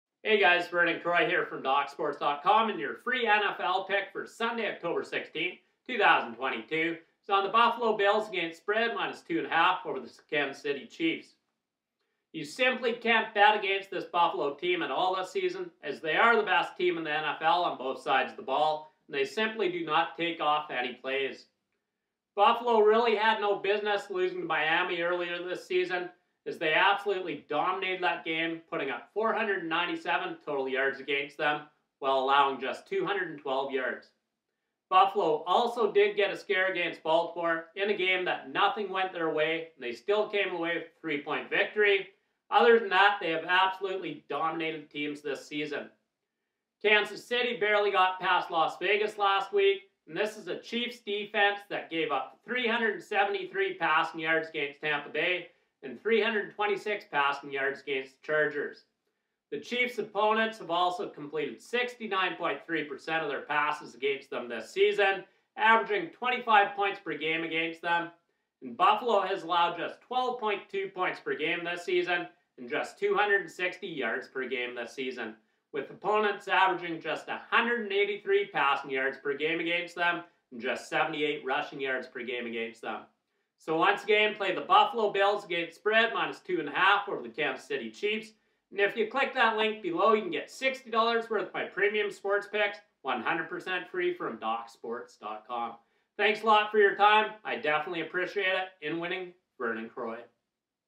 NFL Picks - Buffalo Bills vs Kansas City Chiefs Prediction, 10/16/2022 Week 6 NFL Expert Best Bets
Kansas City Chiefs vs Buffalo Bills 10/16/2022. The Buffalo Bills travel to Kansas City, MO to face the Kansas City Chiefs at 4:25PM EST at GEHA Field at Arrowhead Stadium.

Doc's Sports provides NFL picks against the spread, expert predictions, betting tips and NFL odds on this matchup of Kansas City Chiefs vs Buffalo Bills.